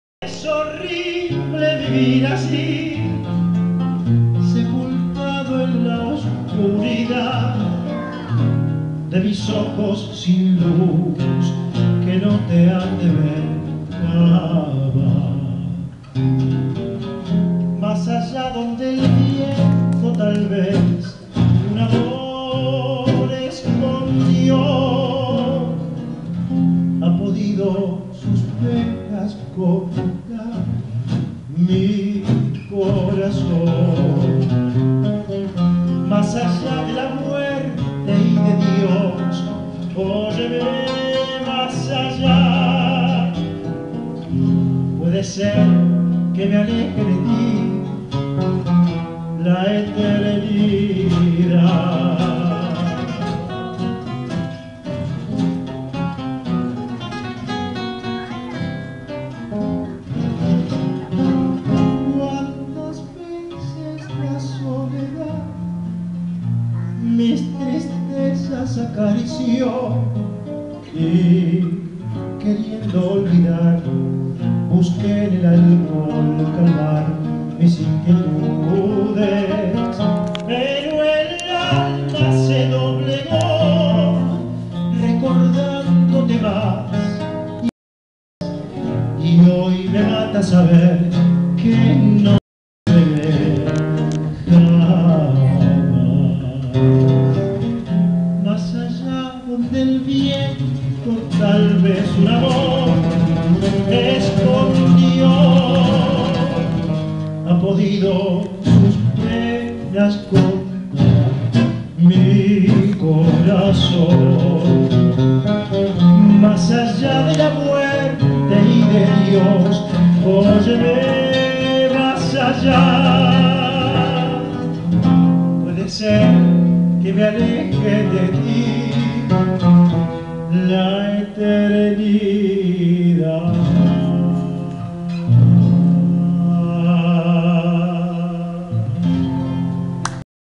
Martin Alvarado en Buenos Aires- Junio 2012-I
Asistiendo al recital ofrecido por Martín Alvarado junto a geniales músicos en el teatro "El popular" de CABA.
Una maravilla.